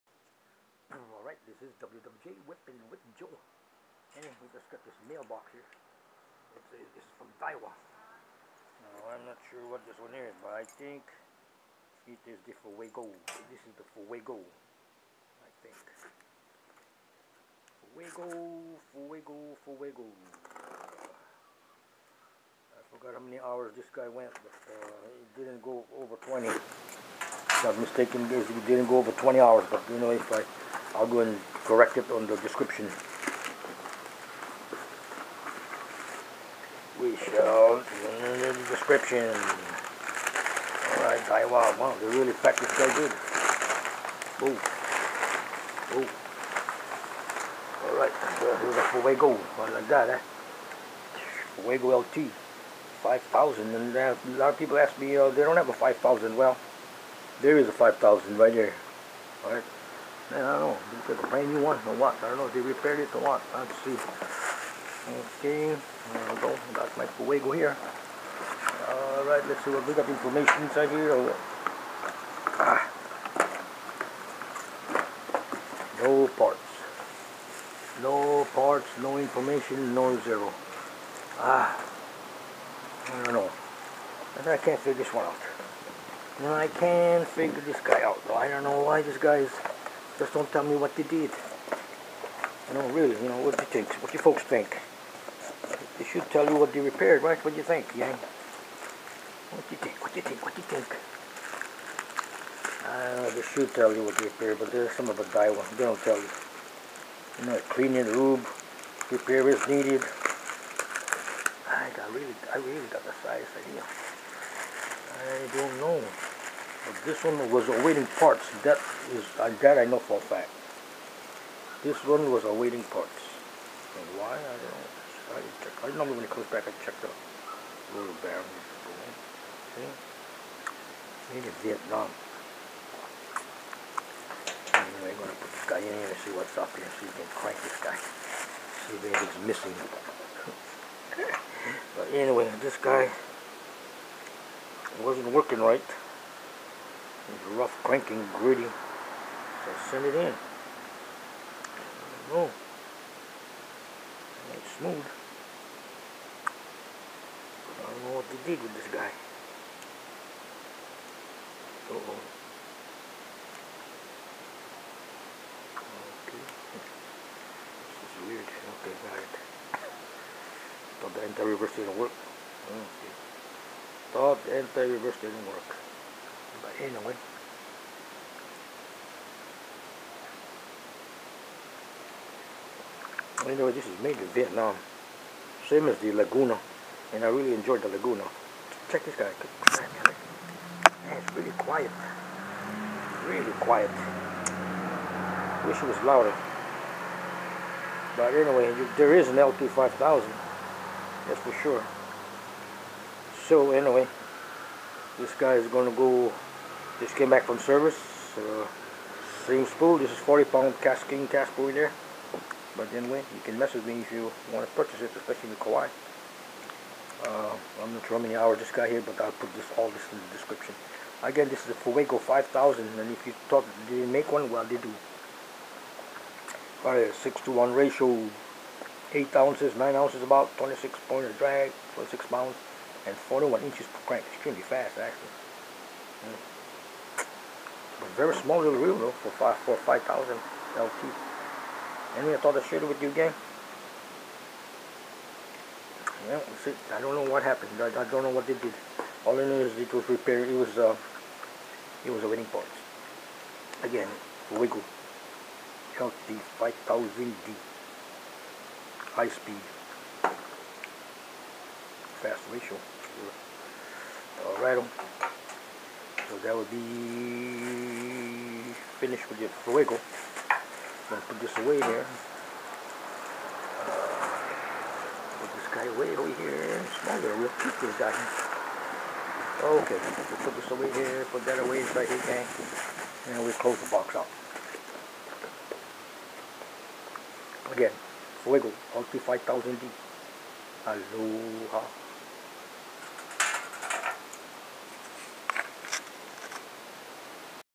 Daiwa Fuego LT5000D CXH Repaired
wwj whipping with joe, back from repair after cranking and popping 2oz lures failed in 11 hours cranking rough.  It was in repair 7 jul, 12 jul awaiting parts got it today 11 Sep 2021.
Thanks to Daiwa for their service..only problem so far is when I get it back their is no record of what they did to repair it..I know it needed  part.  But dont know what they repaired, pinion, drive or maybe pinion gear? Would be nice to get a record of the repair..come on Daiwa!!